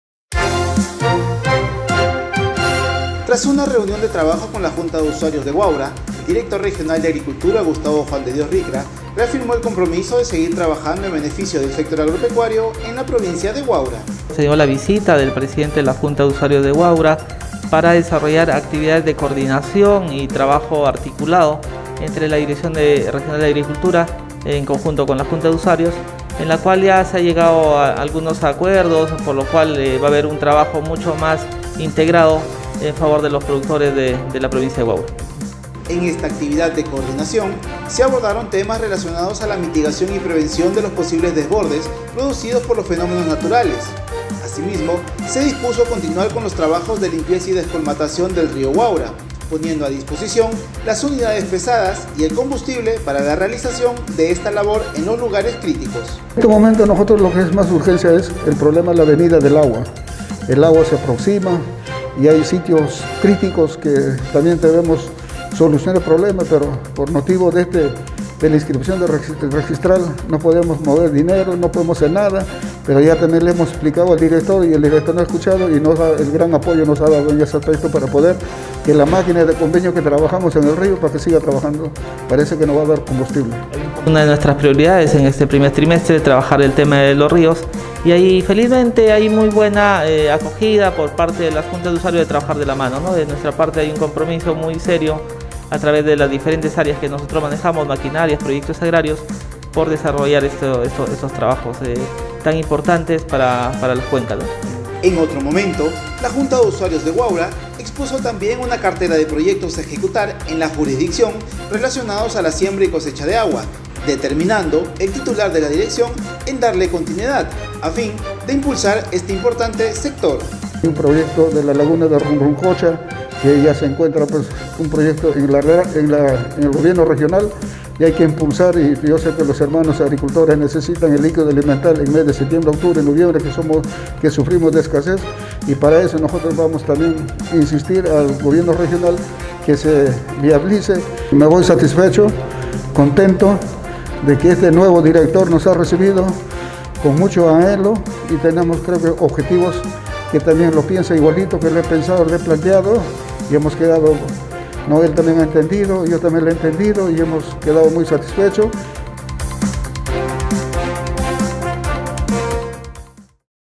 DIRECTOR DE LA DRAL REAFIRMA COMPROMISO EN LOS TRABAJOS DE MITIGACIÓN Y PREVENCIÓN EN EL RÍO HUAURA
DIRECTOR DE LA DRAL JUAN DE DIOS RICRA REAFIRMA COMPROMISO EN LOS TRABAJOS DE MITIGACIÓN Y PREVENCIÓN EN EL RÍO HUAURA
✅ Tras una reunión de trabajo con la Junta de Usuarios de Huaura, el director regional de Agricultura, Gustavo Juan de Dios Ricra, reafirmó el compromiso de seguir trabajando en beneficio del sector agropecuario de la provincia de Huaura.
📌 El presidente de la Junta de Usuarios Huaura, Valerio Rivera García, resaltó la predisposición de la actual gestión regional, articulando compromisos que en conjunto beneficien a los productores de esta zona de la región Lima.